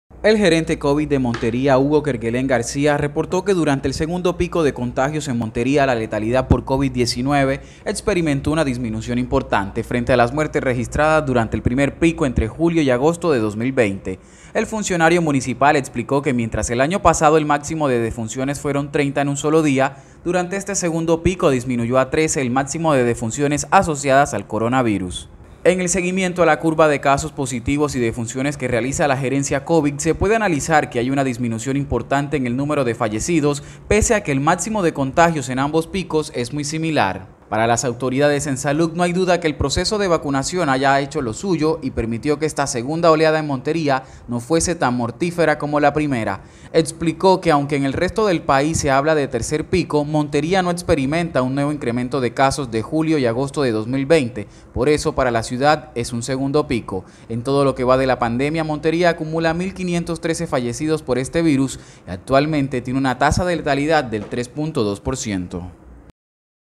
‘En esta segunda oleada experimentamos disminución en la mortalidad’: gerente covid
El gerente Covid de Montería, Hugo Kerguelén García, reportó que durante el segundo pico de contagios en Montería la letalidad por COVID-19 experimentó una disminución importante, frente a las muertes registradas durante el primer pico entre julio y agosto de 2020.

¡Siempre Informando! 📢